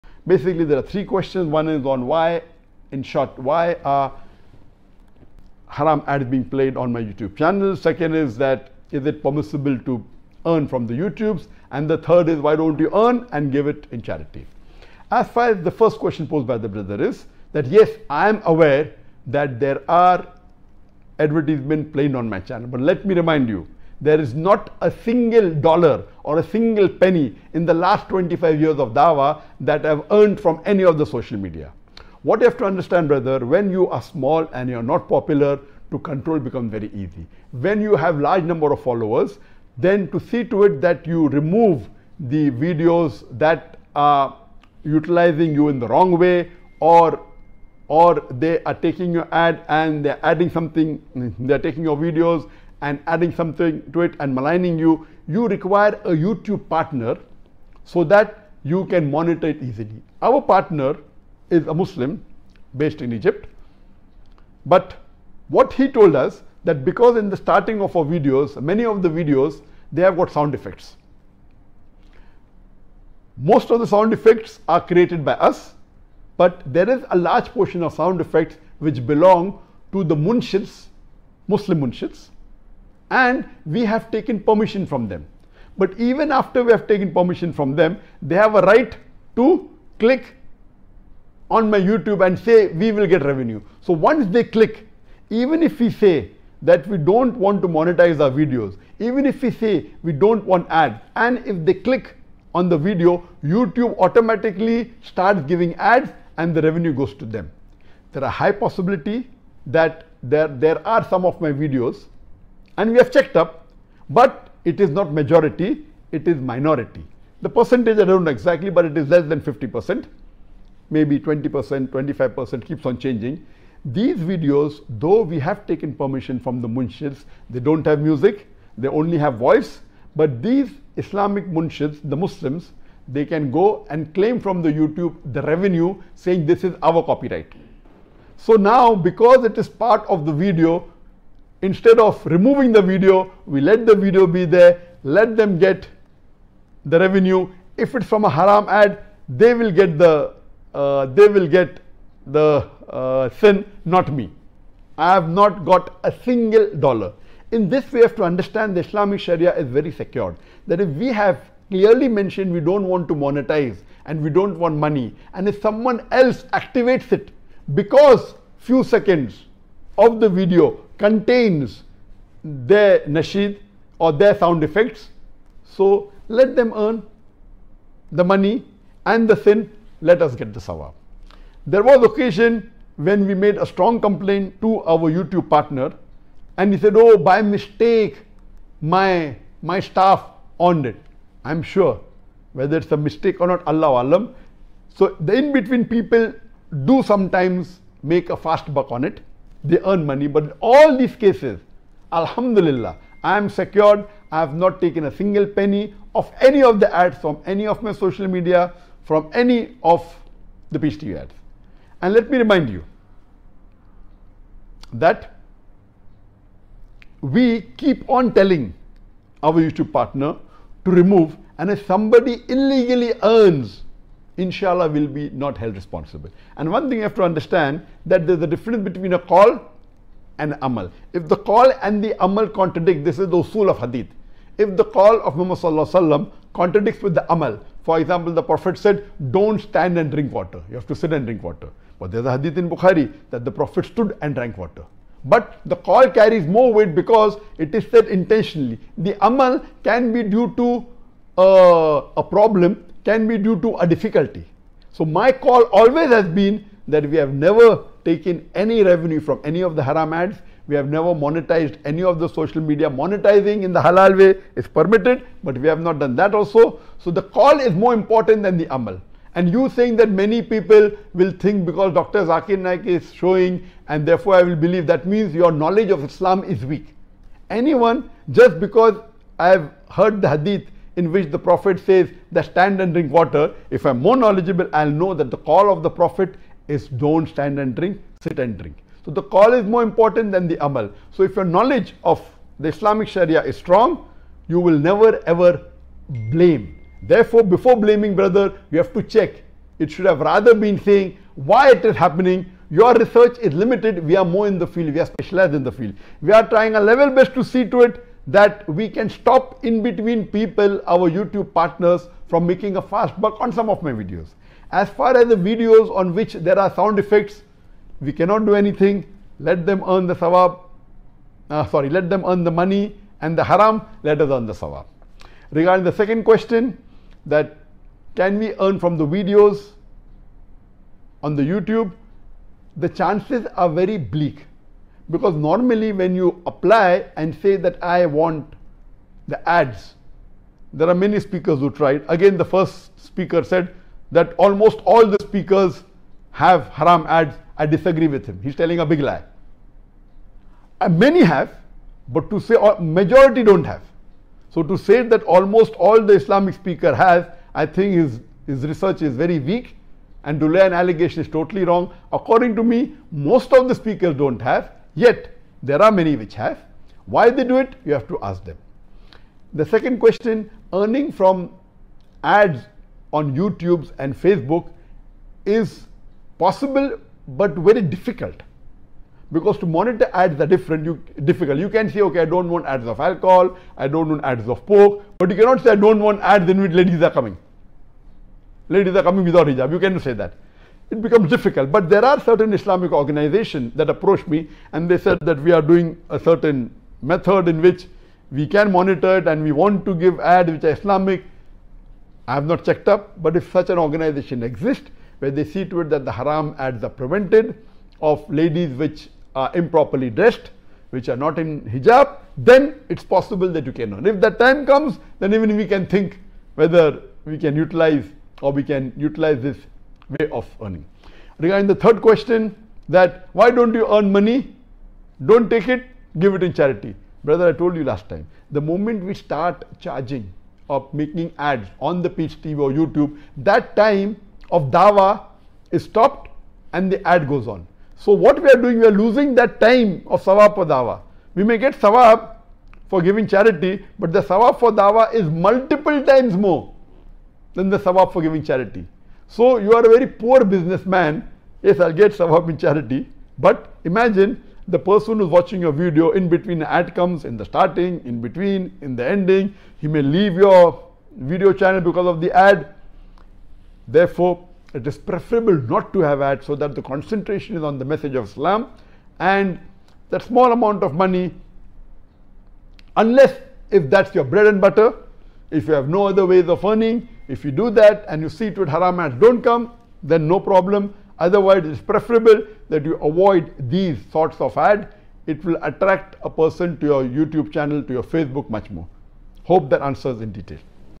Earning from Youtube is Halal or Haram? by Dr Zakir Naik |youtube earning in islam | new video
Earning from Youtube is Halal or Haram? by Dr Zakir Naik |youtube earning in islam | youtube earning